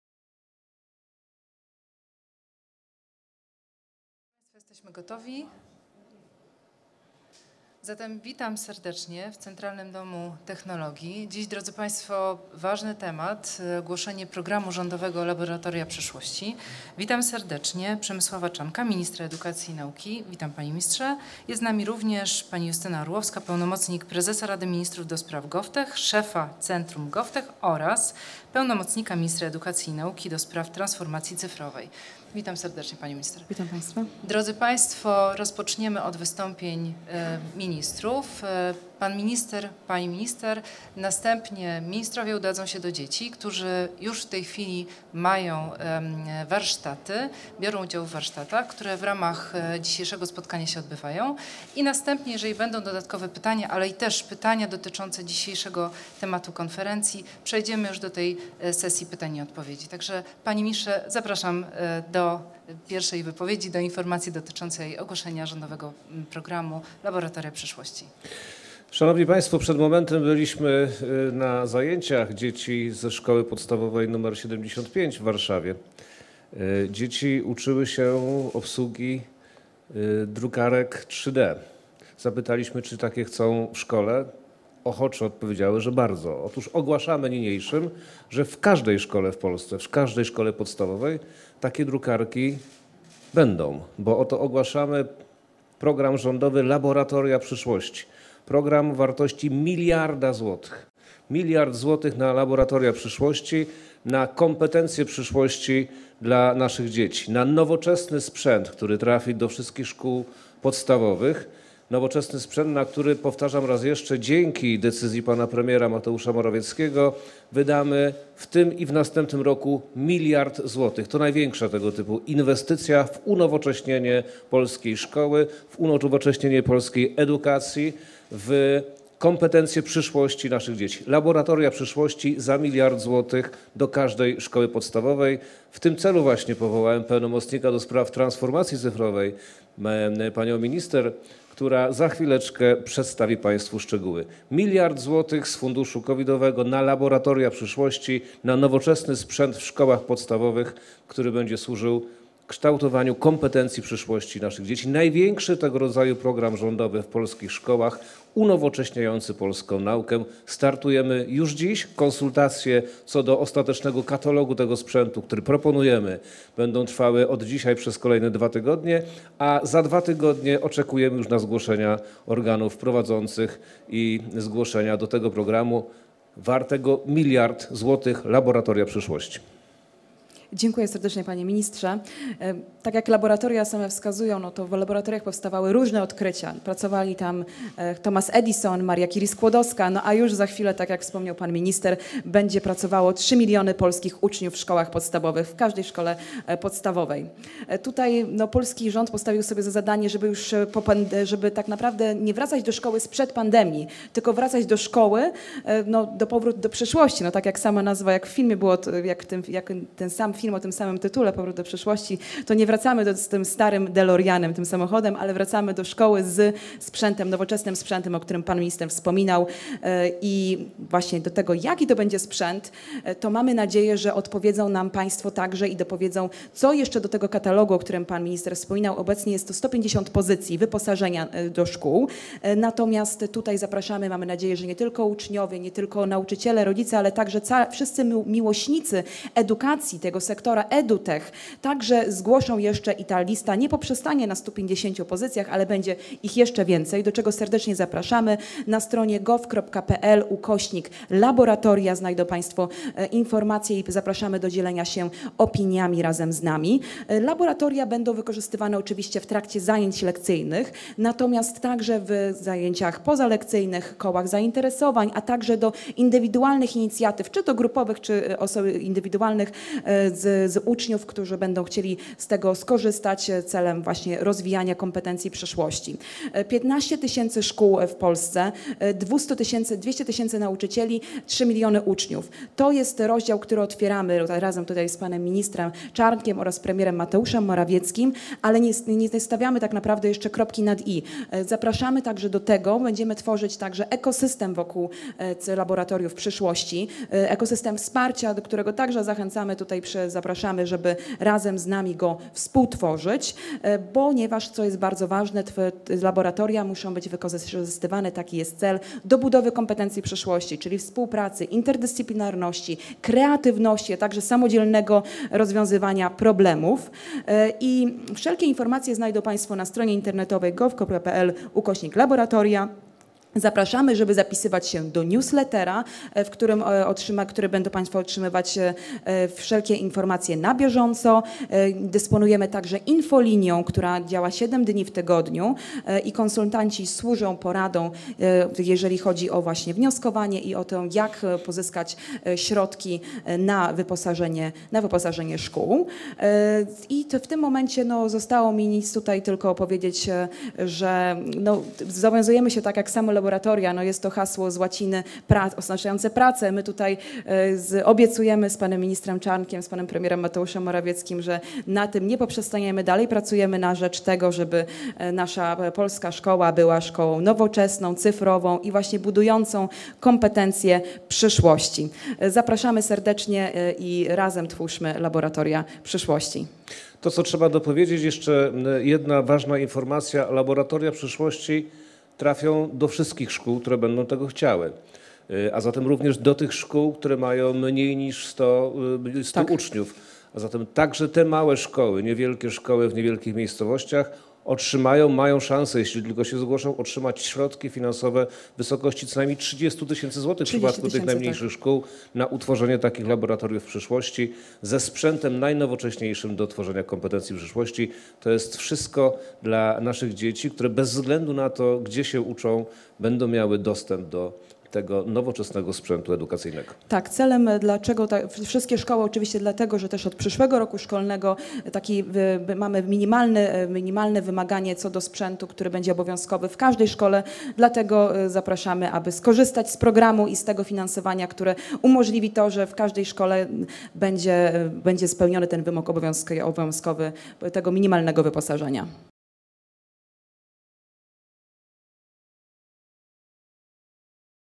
Laboratoria Przyszłości – konferencja z udziałem Przemysława Czarnka i Justyny Orłowskiej.
Zapraszamy do obejrzenia konferencji prasowej dotyczącej Laboratoriów Przyszłości i posłuchania o największej inwestycji w technologie edukacyjne w historii państwa polskiego.

Organizatorami są Ministerstwo Edukacji i Nauki oraz Centrum GovTech.